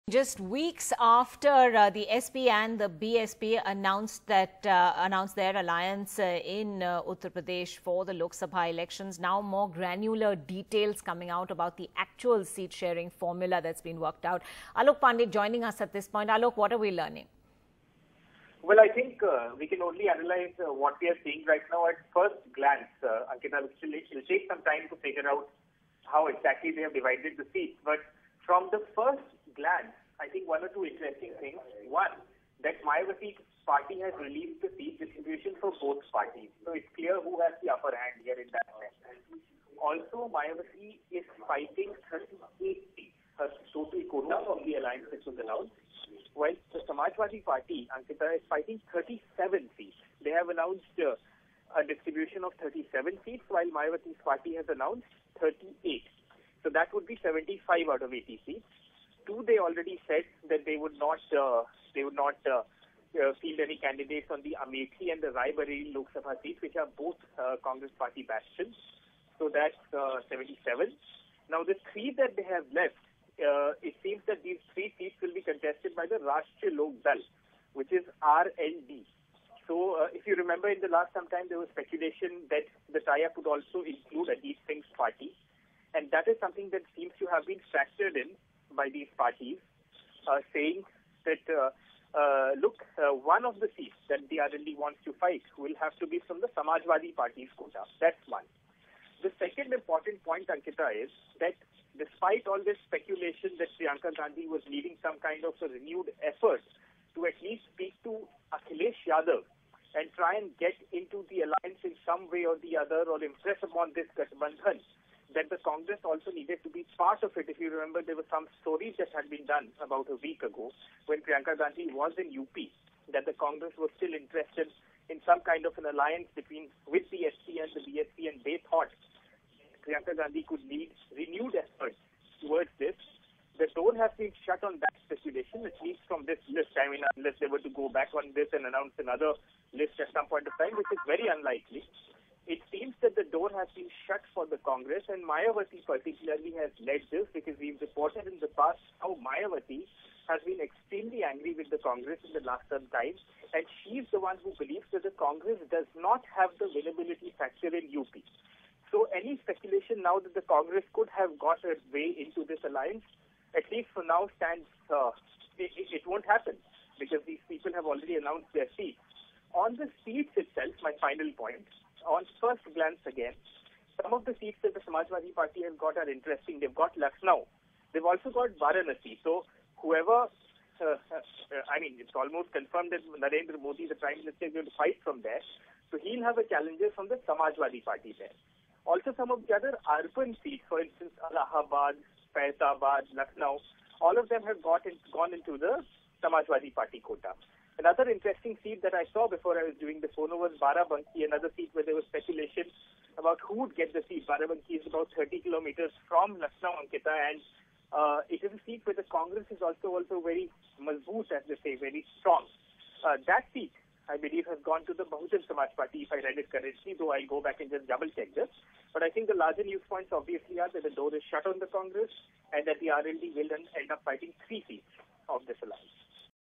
Mayawati, Akhilesh Yadav Announce UP Seat Details, Door Shut On Congress
Ending Congress's hopes for a last-minute berth in the opposition alliance in Uttar Pradesh, Mayawati and Samajwadi Party chief Akhilesh Yadav today shared the final frame of the seat sharing, down to the names. The list had 75 of the state's 80 seats, of which Mayawati's Bahujan Samaj Party gets 38 seats and the Samajwadi Party 37.